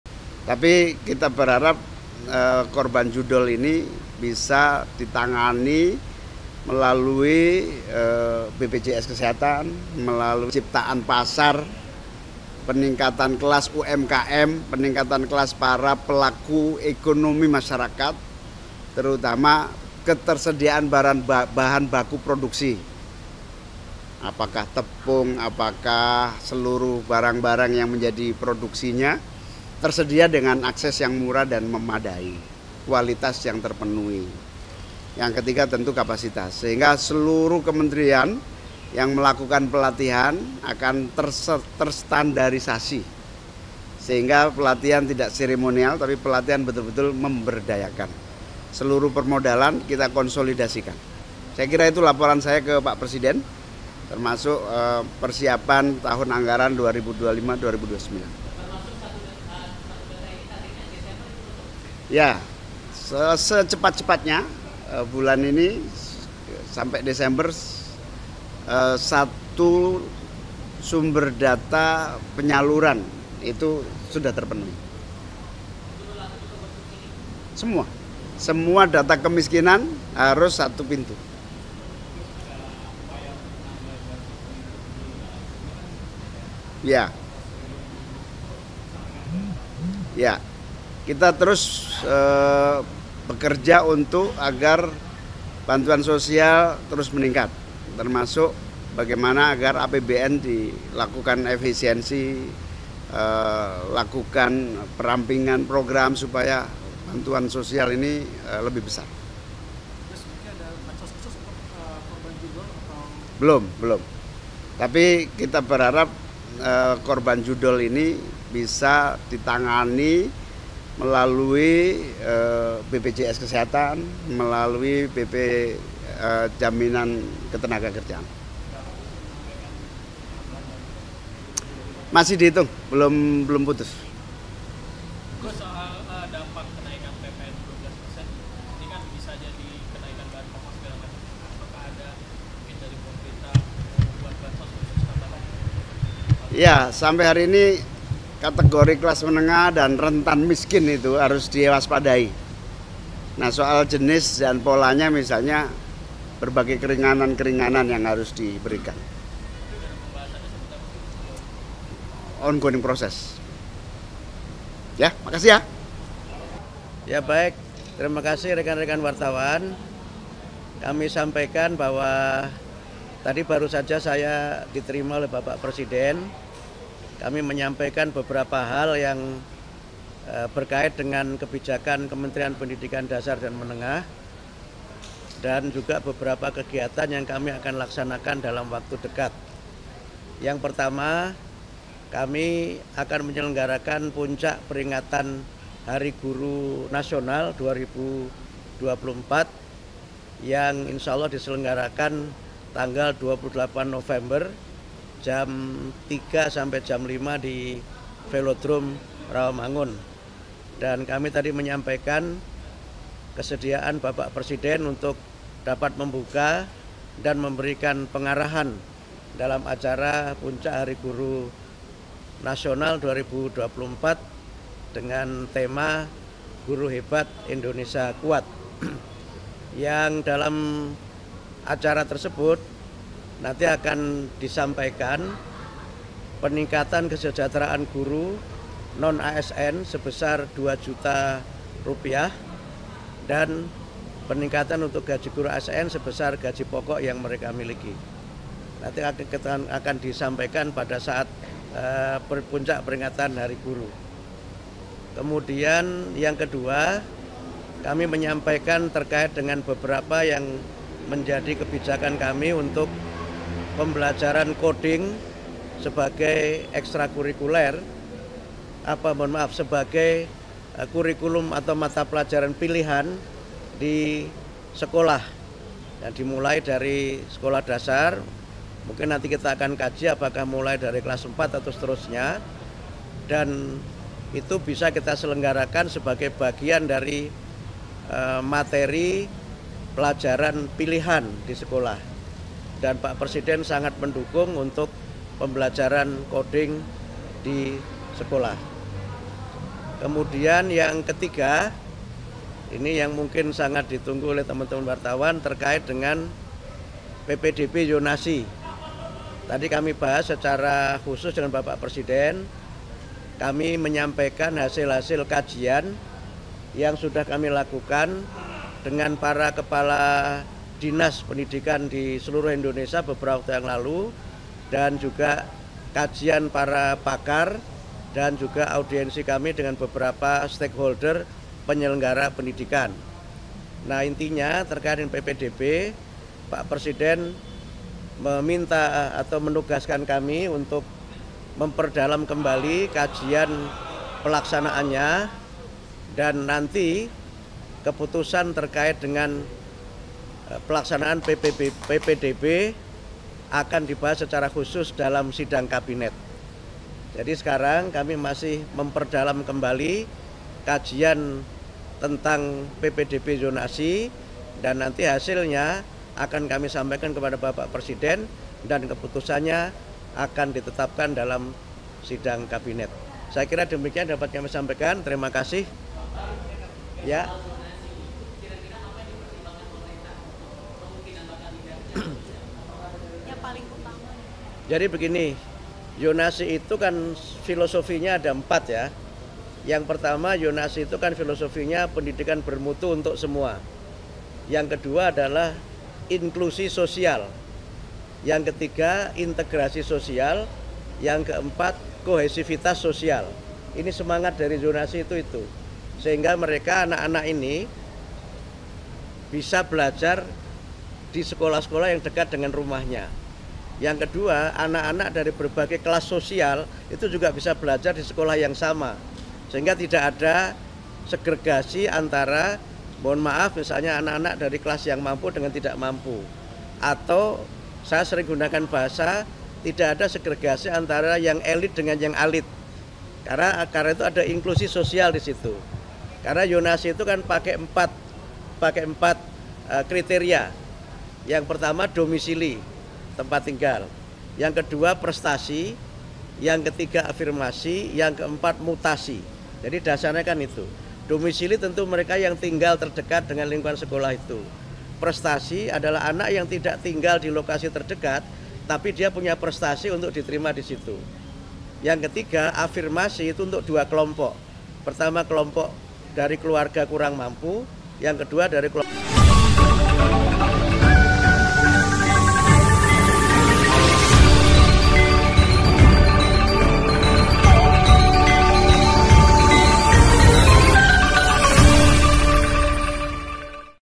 Tambah Kemiskinan Baru, Cak Imin Ingin Bantuan Korban Judol Ditangani Lewat BPJS
Menteri Koordinator Bidang Pemberdayaan Masyarakat (PM), Muhaimin Iskandar, mengharapkan agar bantuan bagi korban judi online dapat dikelola melalui Badan Penyelenggara Jaminan Sosial (BPJS) Kesehatan dan BPJS Ketenagakerjaan atau BPJAMSOSTEK.

"Kita berharap korban judi online ini bisa ditangani melalui BPJS Kesehatan dan BPJS Ketenagakerjaan atau BPJAMSOSTEK," kata Muhaimin saat memberikan keterangan di Istana Kepresidenan Jakarta, Selasa.

Editor